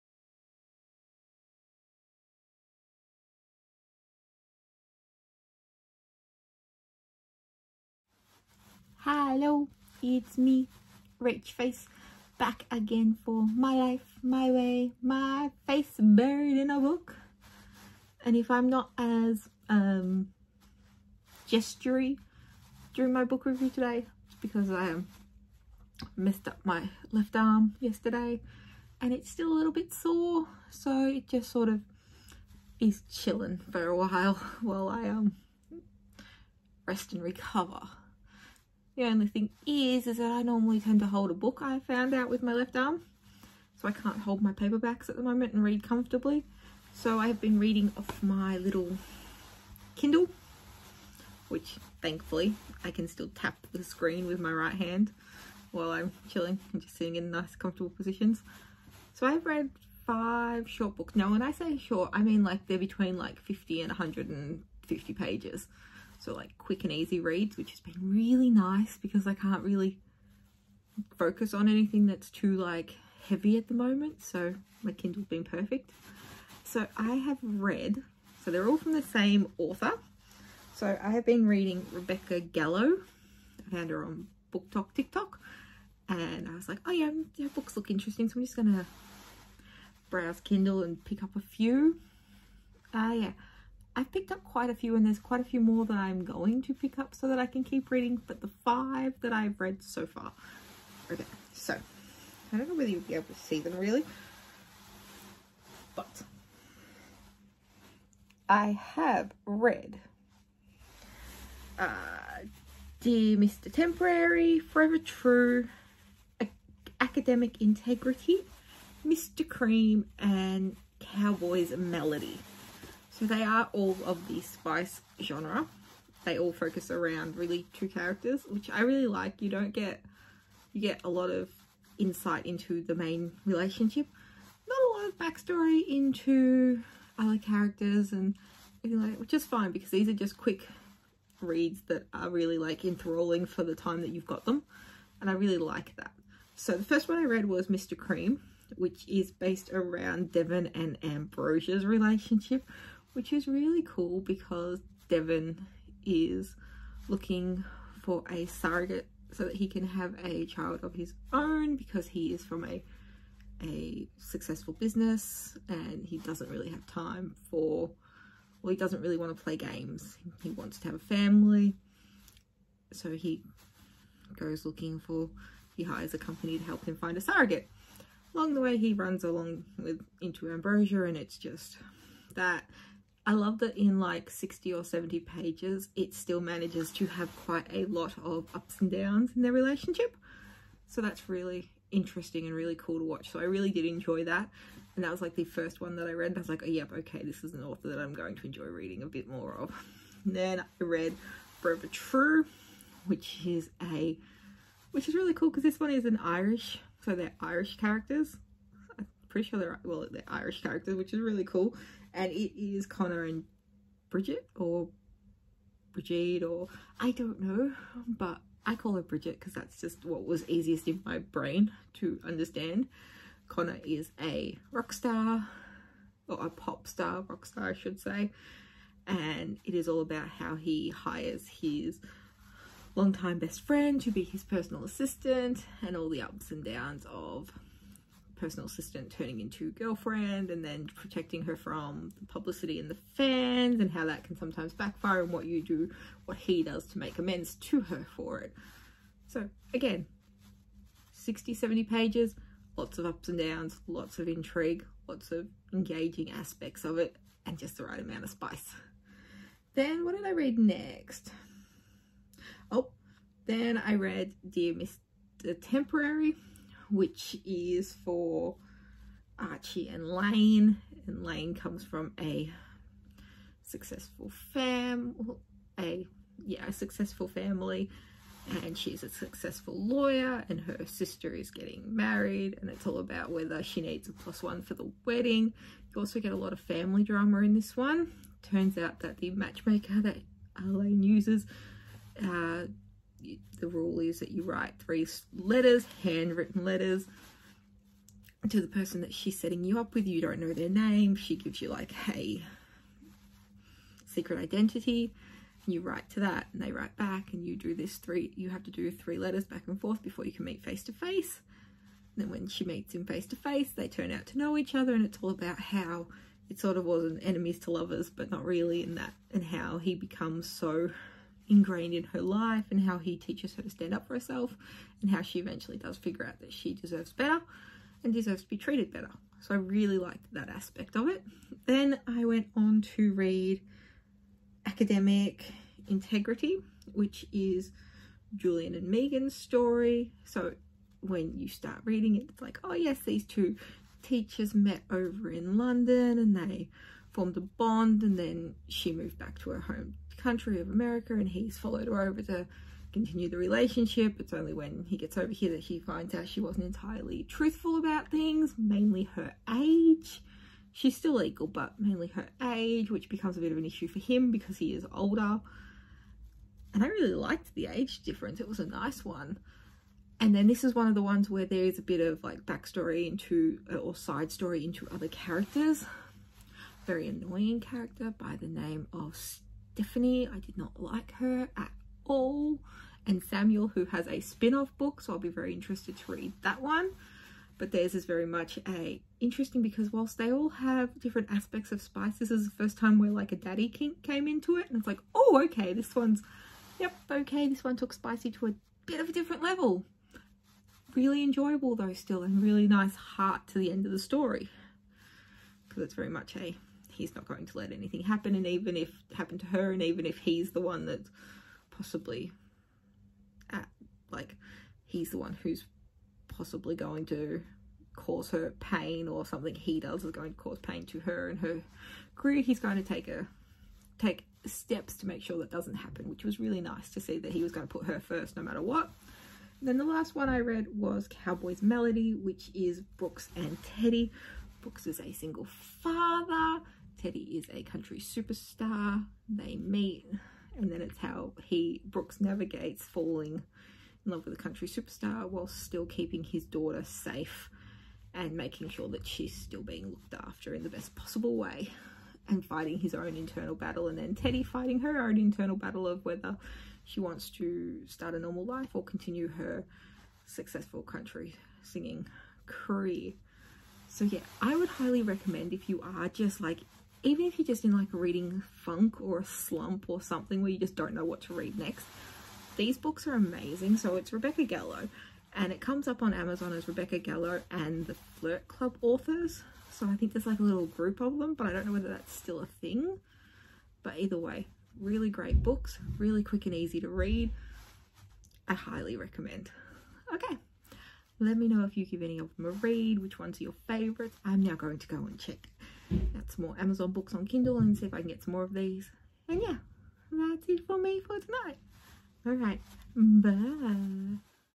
Book Review: Five Quick Reads By Rebecca Gallo
Book Review
Mr Cream, Forever True, Dear Mr Temporary, Academic Integrity, Cowboy’s Melody.
By Rebecca Gallo
18+

Be kind with me, I will always do book reviews a little different. This is simply my opinion, my thoughts and my feelings regarding these books.

One handed reading on my kindle because I have injured my left arm without warning or reason.
So I have found a new author who keeps me engaged and enthralled in short books that have just enough heart, and spice to make a girl forget about the pain in her arm.

Mr Cream: Oh how I wish I could find me a man like Devon, I would even forgive the sins of how we met, but I much like Ambrosia would be more than happy to help him reach his dreams.

Forever True: I would be lying if I said this book wasn’t a dream come true for me. I wish with all my body and soul that I could be Brigid and have grown up being best friends with the gorgeous man that becomes a global superstar. How I want him to hire me and protect me and announce his love for me.

Dear Mr Temporary: Where do I send my handwritten application to Grace Graham, I want nothing more than to have her find my perfect match like she did for Layne and Archie. This takes us back to an easier time when matchmakers worked for us and we didn’t rely on Tinder and on-line dating.

Academic Integrity: Imagine a summer romance in a foreign country, imagine heading home and finding out your summer love has come to find you, except you may have fibbed about your age and that turns out to be a big no no for your lover. A gorgeous age gap and a daddy kink to leave you wanting more.

Cowboy’s Melody: Here comes a role reversal for you, single cowboy father, female country superstar. Takes learning to love and learning to make room and figure out that you can have it all, you just need to take a chance every now and again. Push yourself to your limits as you find out a little more about yourself and your family.

My review might be a little all over the shop but I will always fill it with what I liked and a base overview before delving into why I liked or disliked or in this case loved a novel.